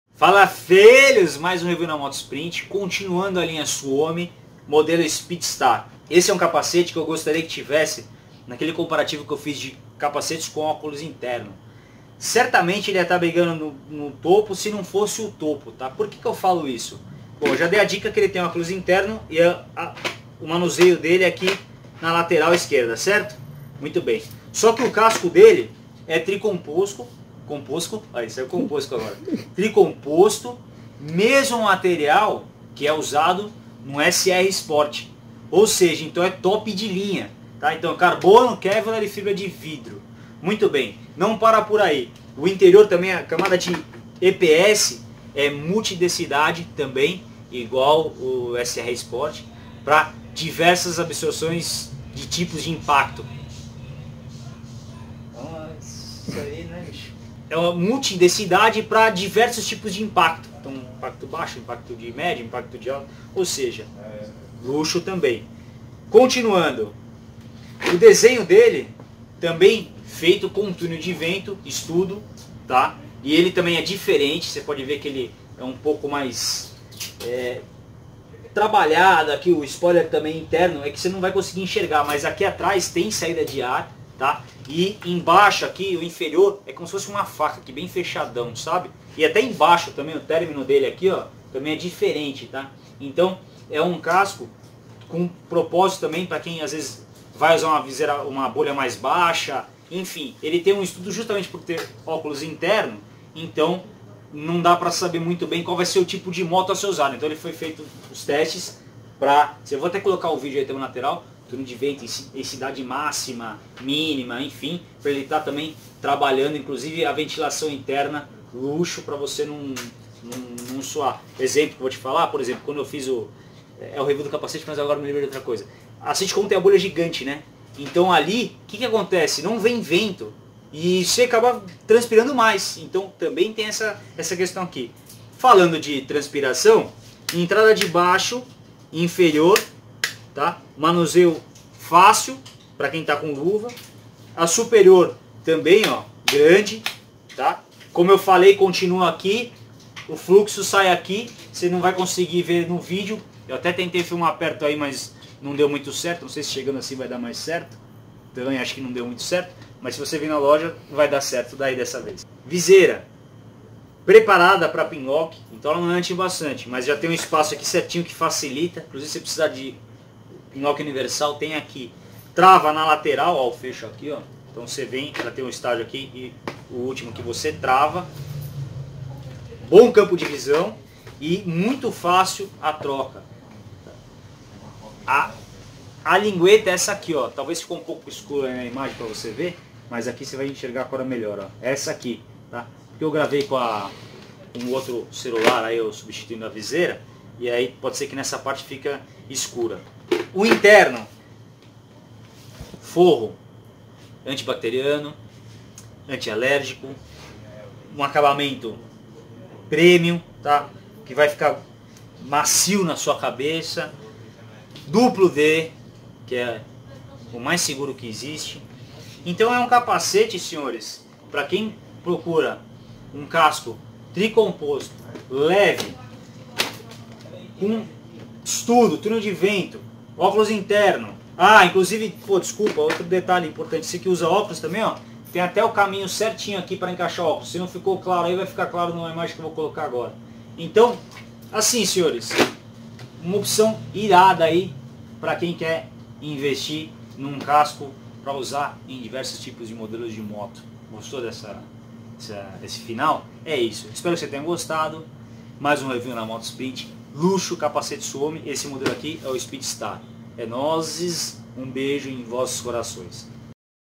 REVIEW DO CAPACETE SUOMY SPEEDSTAR - Motosprint
Suomy Speedstar, casco tri - composto na mesma pegada do SR Sport, EPS interno com densidades diferentes para melhor absorção de impacto, óculos interno e espaço para você que usa óculos de grau.